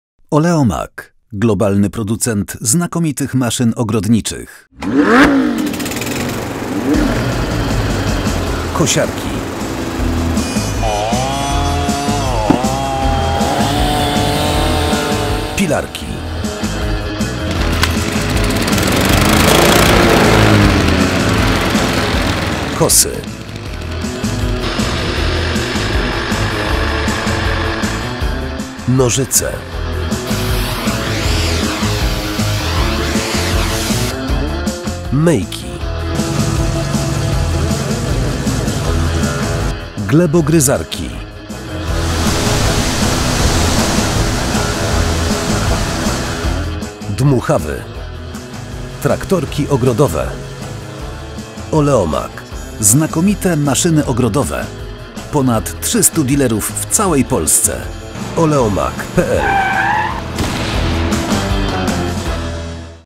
Poznaj asortyment maszyn Oleo-Mac
Producentem urządzeń Oleo-Mac jest firma Emak posiadająca 50 lat doświadczeń w produkcji maszyn dla ogrodnictwa i leśnictwa. Produkty marki Oleo-Mac to najwyższej jakości urządzenia, które są w stanie zadowolić nawet najbardziej wymagających użytkowników, zarówno właścicieli ogrodów jak i profesjonalistów, dbających o zieleń zawodowo.